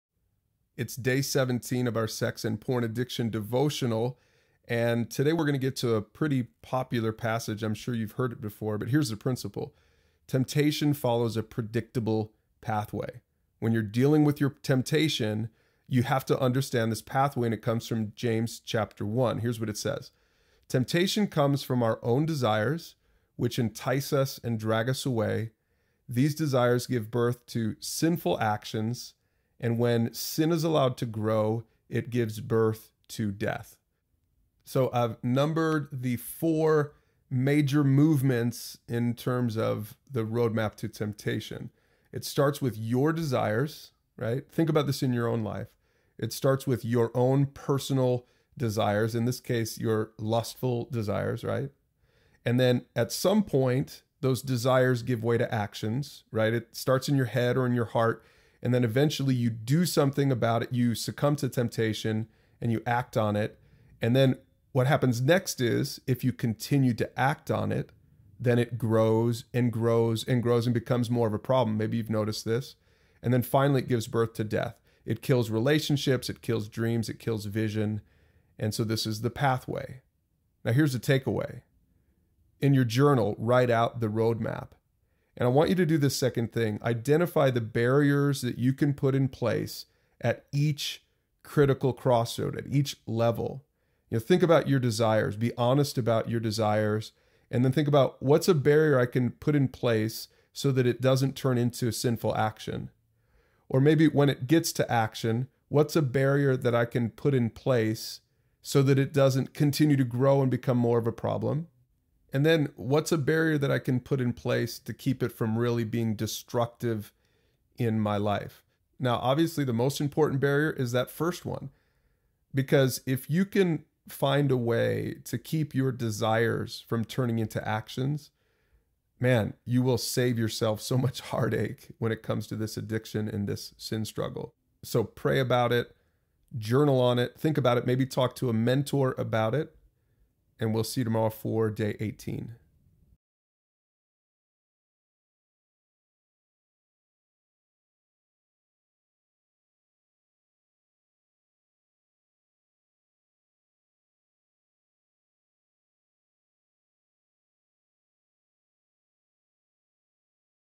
Roadmap To Temptation
Sexual temptation follows a predictable pathway in our lives. If we can identify that pathway, we can set up barriers to keep Temptation from leading to death in our lives.